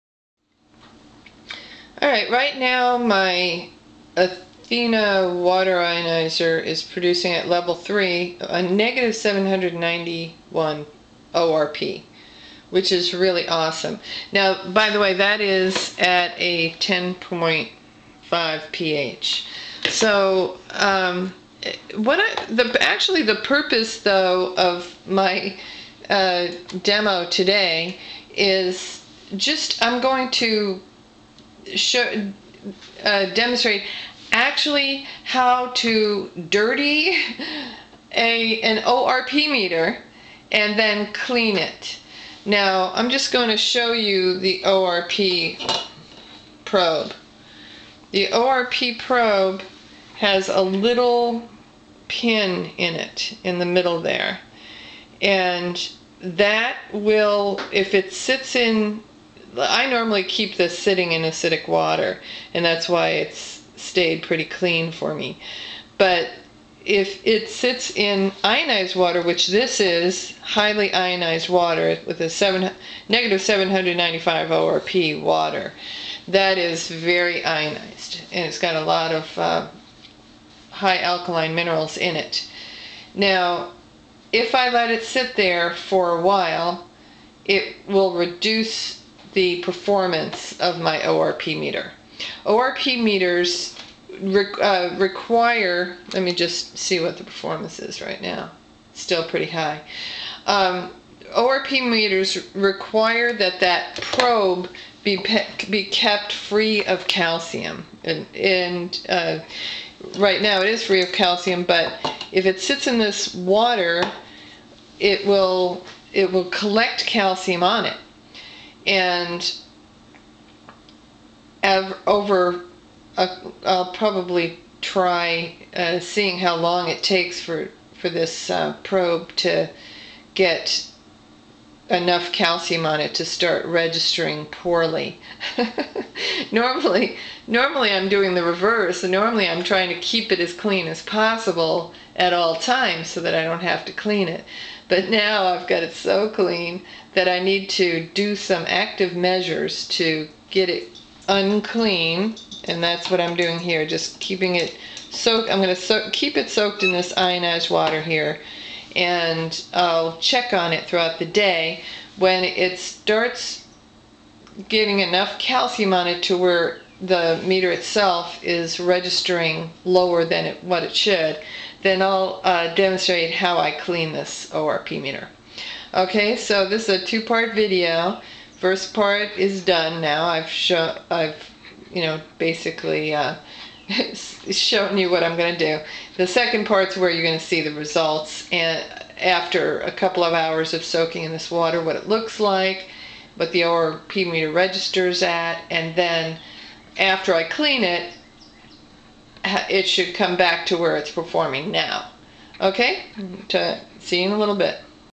How-to-clean-an-ORP-meter-1
, is demonstrating a simple and effective system [which she has developed over several years of working with water ionizers] for cleaning the very stubborn and hard-to-condition ORP meter. Most people don't need to worry about getting or learning how to use their own ORP meter to test how many anti-oxidants can be expected in every glass of ionized water. Cathie has done all these tests for you. However if you are one of those people who likes testing your own ionized water with your own ORP meter, then this video is a must-see. You WILL get inacurate readings unless you know how to properly clean your ORP meter.